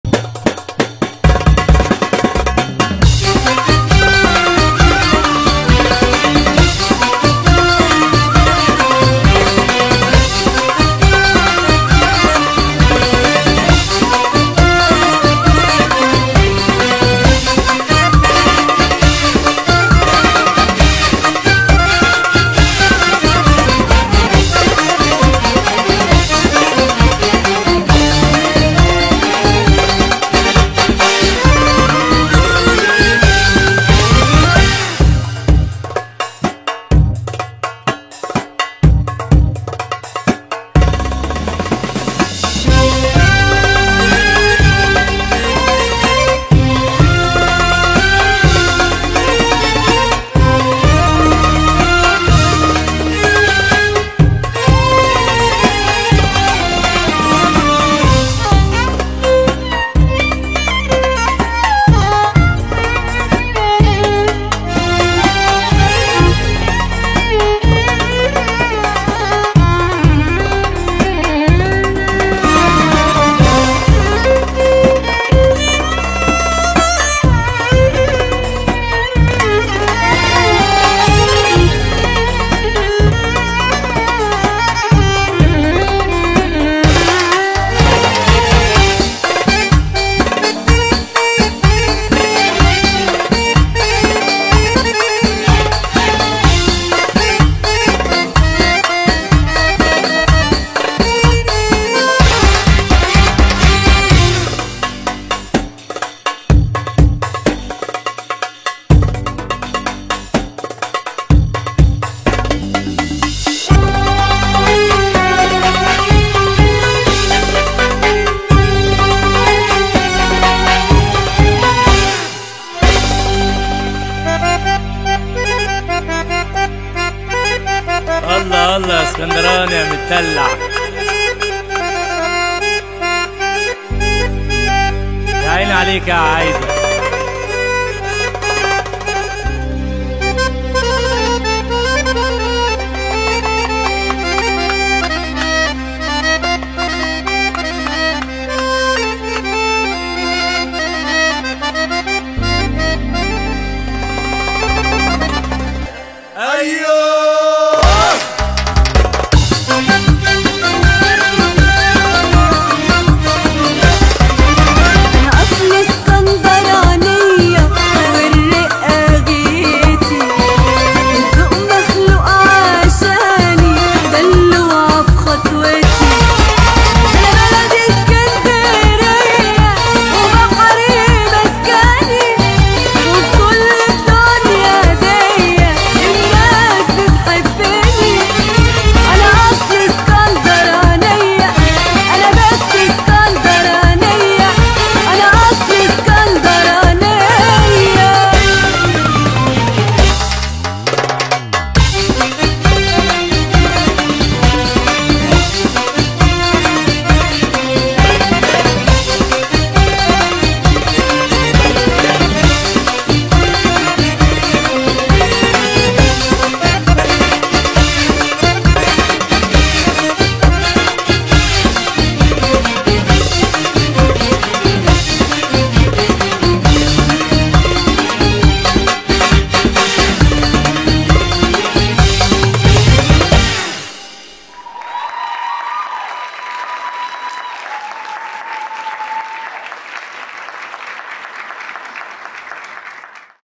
Rita Pereira - Aida Bogomolova Mejancé | Professional Category | GODance Festival
My performance in the Professional Category in GODance Festival in Guimarães, Portugal.

Judges: Dariya Mitskevich, Diana Gnatchenko, Wael Mansour & Nesma Al-Andalus